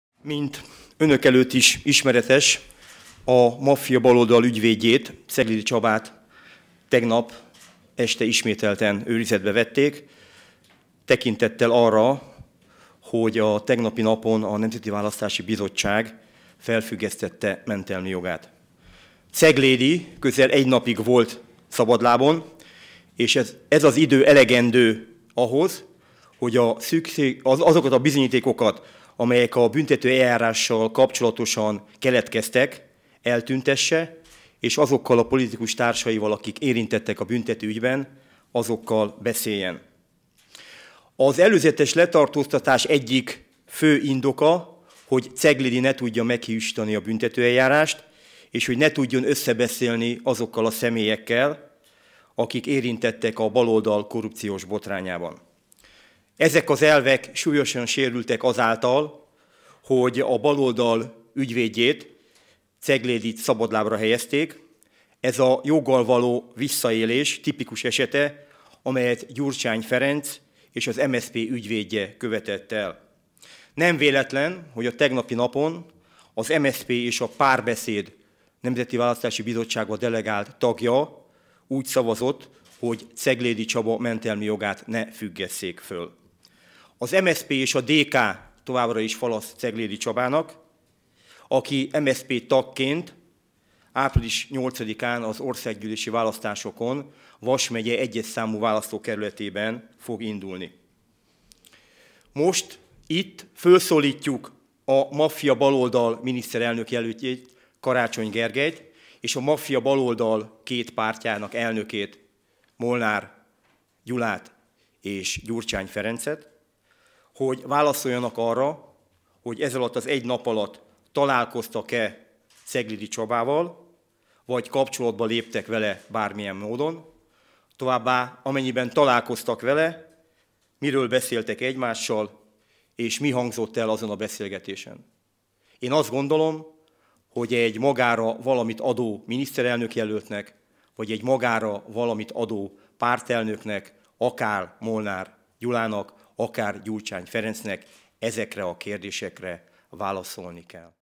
Kapcsolatba lépett-e Czeglédyvel Karácsony, Gyurcsány vagy Molnár?
A Fidesz felszólítja a maffiabaloldal miniszterelnök-jelöltjét, Karácsony Gergelyt, továbbá a maffiabaloldal két pártjának elnökét, hogy válaszoljanak arra, találkoztak-e, vagy bármilyen módon kapcsolatba léptek-e Czeglédy Csabával, míg szabadlábon volt – jelentette ki Budai Gyula szombati sajtótájékoztatóján.